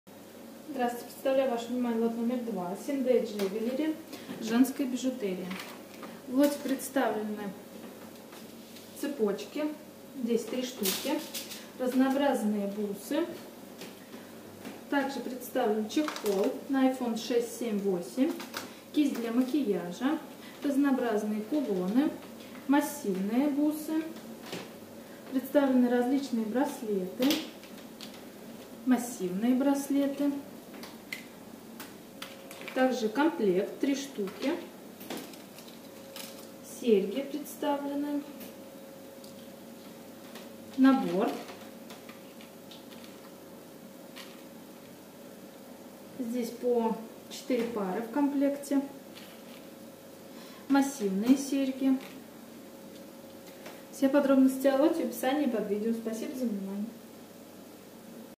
C&A JEWELLERY (Лот№ 2) Цена за кг : 13.31 евро, сток одежда оптом
C&A JEWELLERY (Лот№ 2)
Бижутерия женская (сток)
Страна: Германия
Демисезон
Цена за мешок: 13.31 евро
Цена за кг: 13.31
Цена за ед.: ~ 0.26  евро
Вес мешка: 1  кг